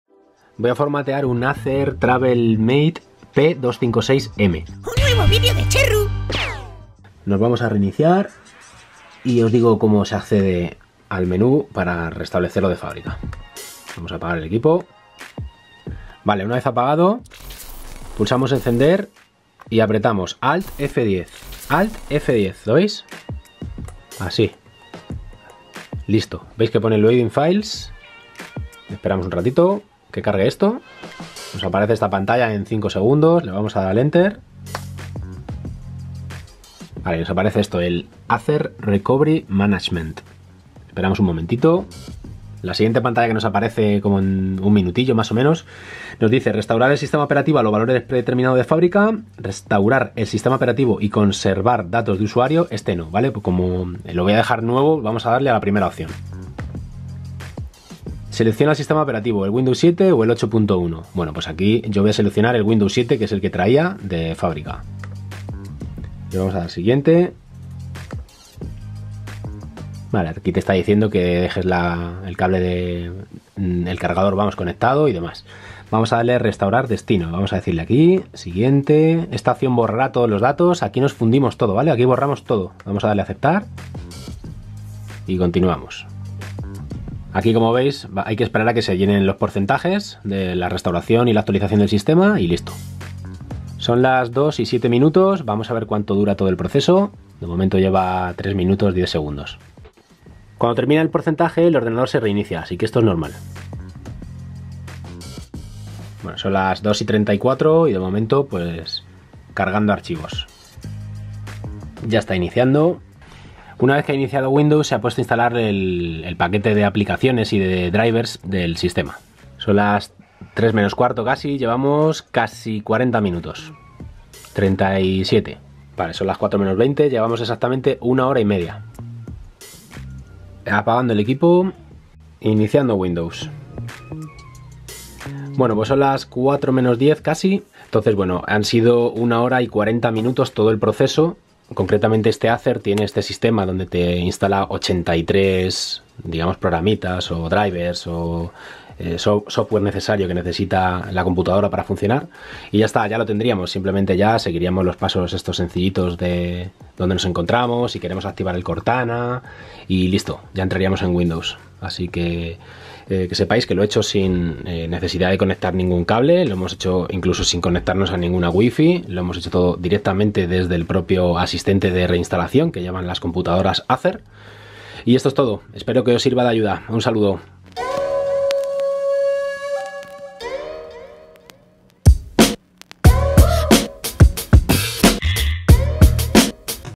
FORMATEAR PORTÁTIL ACER | Sistema sin CD ni drivers
Sistema fácil y rápido para formatear un portátil ACER y devolverlo a su estado de fábrica.

ACTUALIZO:
► Únete a mi canal de Telegram ► Descarga todo gratis en mi Discord